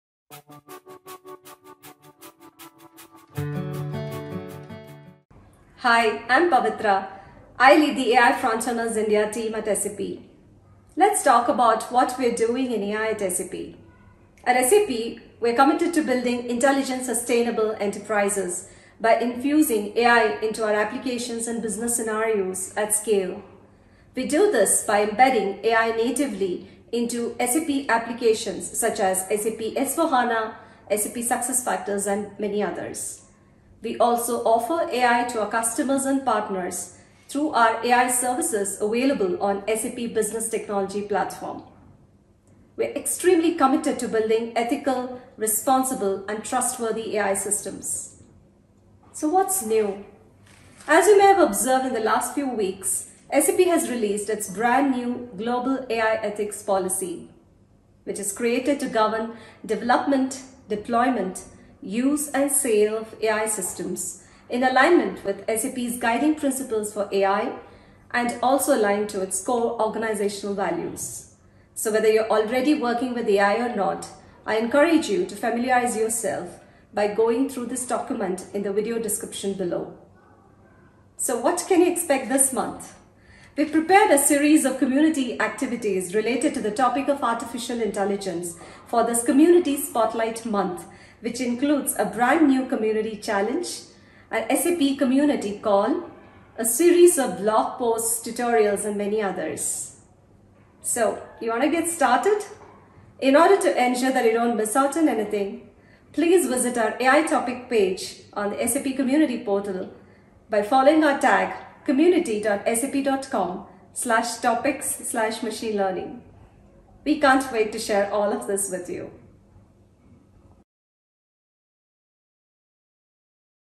Artificial Intelligence in the SAP Community
Pavithra G K takes us through upcoming events and challenges in the SAP Community around Artificial Intelligence.

0:00 Introduction
0:14 What we are doing in AI at SAP
0:48 What is new in AI at SAP?
1:20 What can you expect this month?